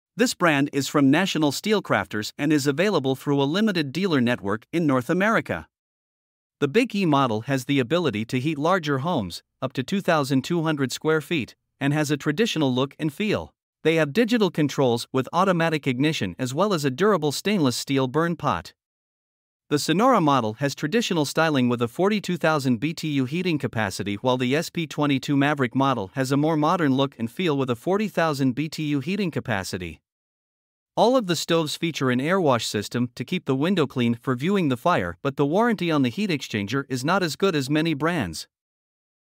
HvacRepairGuy 2023 Breckwell Brand Pellet Stove Reviews
In this video, HvacRepairGuy reviews the 2023 Breckwell brand pellet stoves. These reviews are for the Big E, Sonora, & SP22 Maverick models.

You can get the reviews of all of the brands